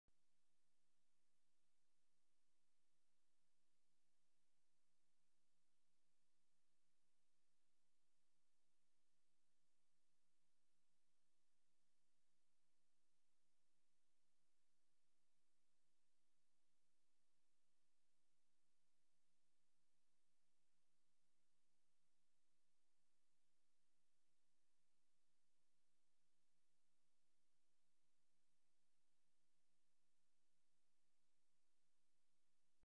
GREEK GOVERNMENT - NO SOUND
Exterior building. Members of Parliament. Mr Panagiotis Pipinelis. Speeches.